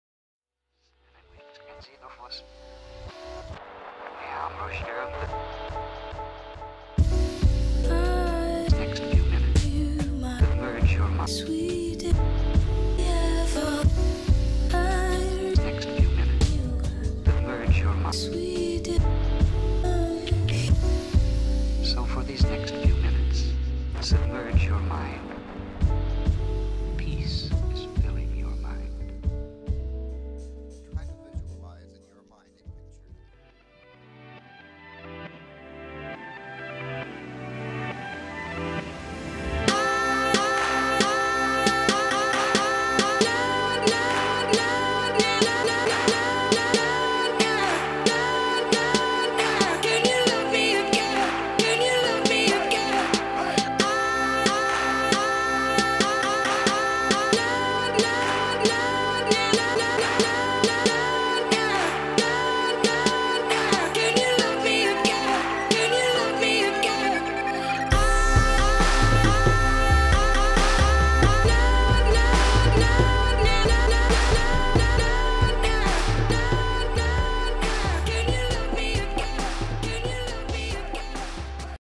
Him and Her(A short Fashion film)
This is a short fashion film. It was fun to shoot and even more of a joy to edit. I shot it on a Sony A7S II in a very flat picture profile and later graded it . I believe we got some great results